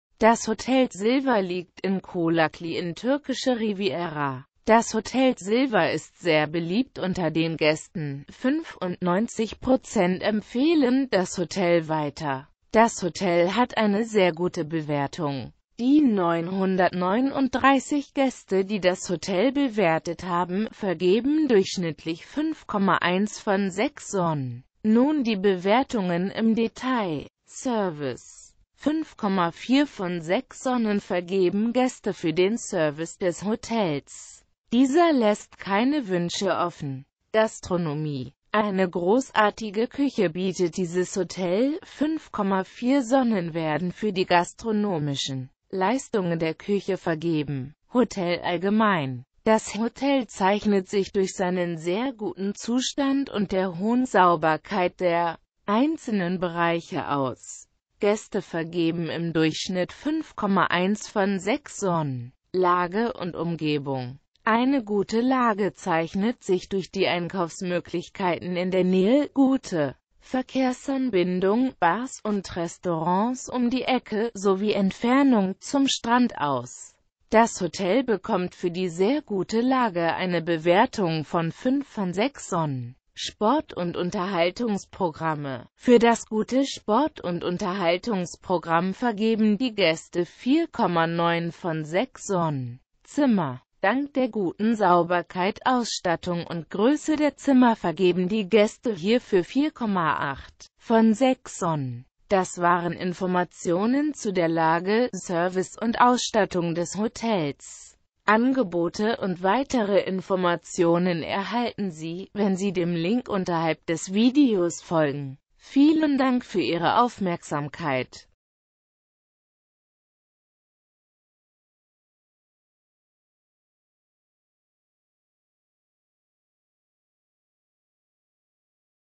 Hotel Silver in Colakli (Tuerkische Riviera - Tuerkei) Bewertung und Erfahrungen
Das Hotel Silver liegt in der Stadt Colakli welche zu Tuerkei (Europa) gehoert.

 Das Hotel Silver ist sehr beliebt unter den Gaesten, 95% empfehlen das Hotel weiter. Zudem erhaelt das Hotel 5.1 von 6 Sonnen.
Service:
 5,4 von 6 Sonnen vergeben Gaeste fuer den Service des Hotels. Dieser laesst keine Wuensche offen.
Gastronomie:
Hotel allgemein:
Lage und Umgebung:
 Eine gute Lage zeichnet sich durch die Einkaufsmoeglichkeiten in der Naehe, gute Verkehrsanbindung, Bars und Restaurants um die Ecke, sowie Entfernung zum Strand aus. Das Hotel bekommt fuer die sehr gute Lage eine Bewertung von 5 von 6 Sonnen.

Informationen zur Verfuegbarkeit von Transfer zum Hotel, WLAN in den Zimmern des Hotel Silver, sowie Details zum Animations-Progamm, Entfernung zum Strand oder zu Diskotheken erhalten Sie bei Ihren Reiseveranstalter oder auf der Webseite des Hotels. Neben den hier verwendeten Bewertungen, lohnt sich ein Blick auf die ggf. vorhandene Hotel Silver Facebook-Seite.

Quellenhinweis:
Die verwendeten Informationen stammen von HolidayCheck.de. Sie wurden am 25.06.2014 abgerufen. In diesem Video wurden Bilder aus Colakli oder allgemeine Bilder aus Tuerkei verwendet. Die gezeigten Orte und Landschaften dienen als Beispiel und muessen nicht unbedingt in der Naehe des Hotels liegen.